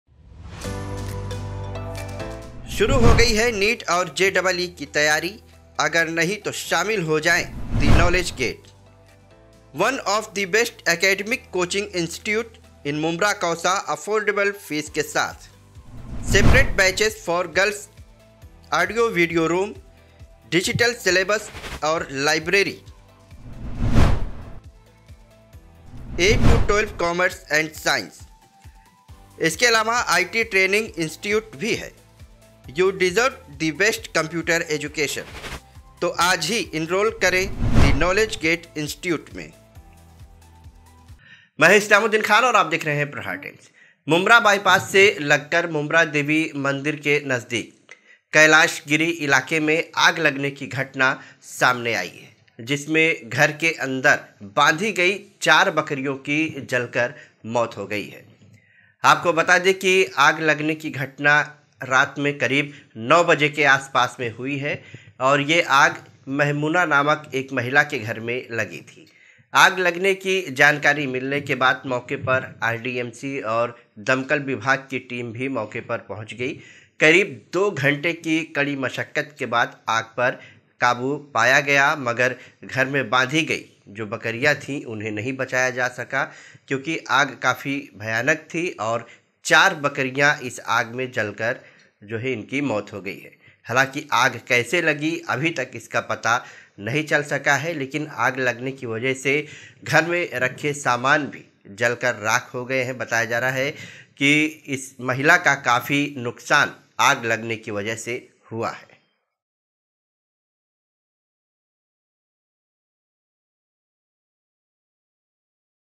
मुंब्रा बाईपास किनारे झोपड़ी में लगी आग,4 बकरियों की जलकर मौत || Islamuddin Khan | Aapka Prahar Times
मुंब्रा बाईपास किनारे झोपड़ी में लगी आग,4 बकरियों की जलकर मौत || Islamuddin Khan

मुम्ब्रा बाईपास से लग कर मुंब्रा देवी मंदिर के नजदीक कैलाश गिरी इलाके में आग लगने की घटना सामने आई जिसमे घर के अंदर बांधी गई चार बकरियों की जलकर  मौत ही गई आपको बता दे आग लगने की घटना रात करीब 9 बजे हुई ये आग महमुना नामक एक महिला के घर मे लगी थी आग लगने की जानकारी मिलने के बाद मौके पर RDMC और दमकल विभाग की टीम भी पहुंची करीब दो घंटे की मशक्कत के बाद आग पर काबू पाया गया मगर घर मे बांधी गई बकरियों को नही बचाया जा सका और चार बकरियों की जलकर मौत ही गई हालांकि आग कैसे लगी अभी तक इसका पता नही चल सका है